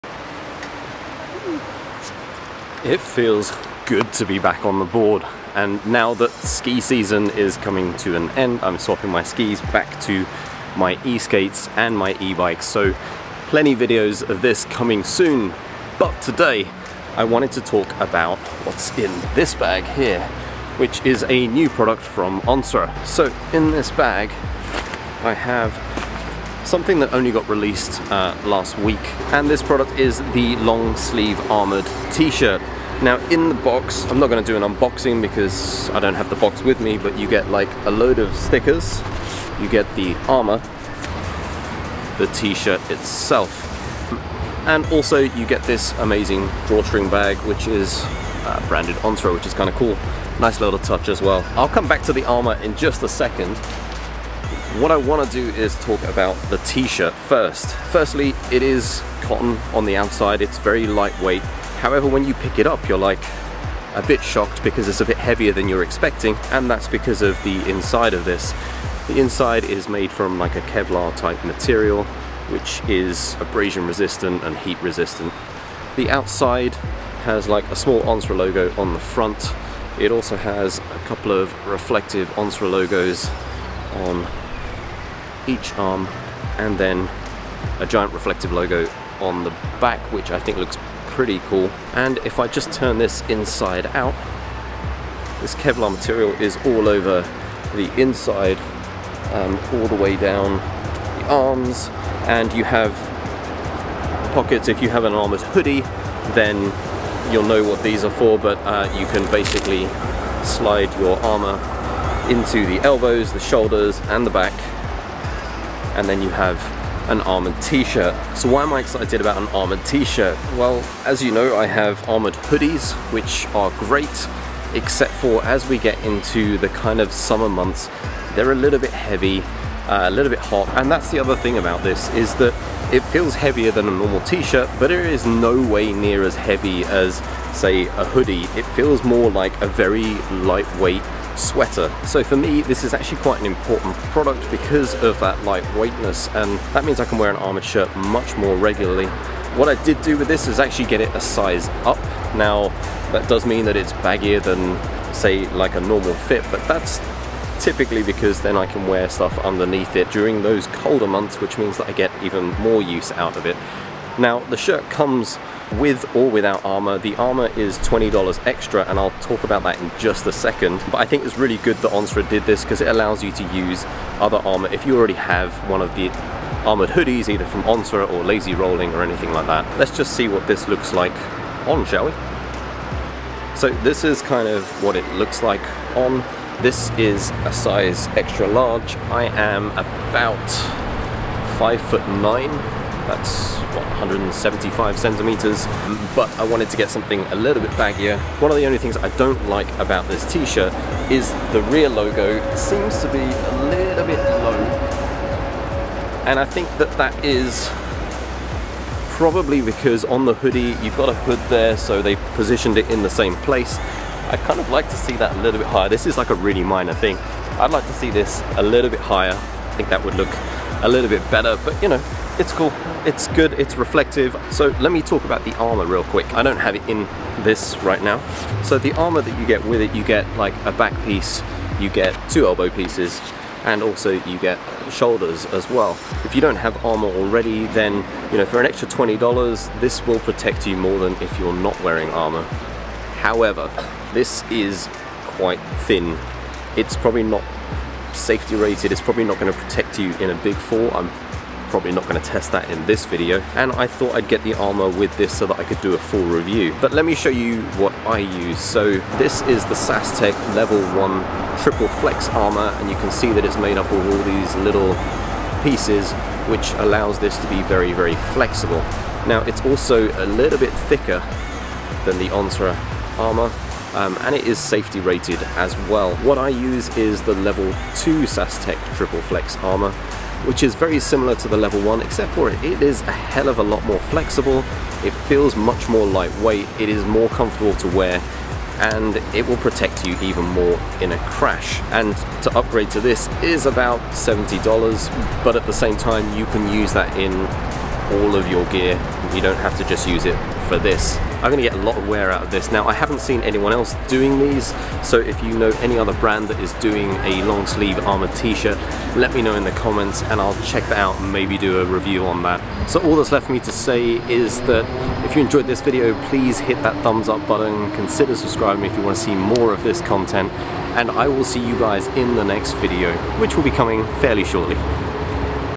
Better than an Esk8 hoodie? Onsra armored long sleeve T-shirt
Today I’m reviewing a new product from @ONSRA , which is their Long-sleeve armoured t-shirt for electric skateboarding.
I have armoured hoodies but can rarely wear them when the weather gets hot. In this video I talk about why I love this, and two small improvements that I would make to this shirt.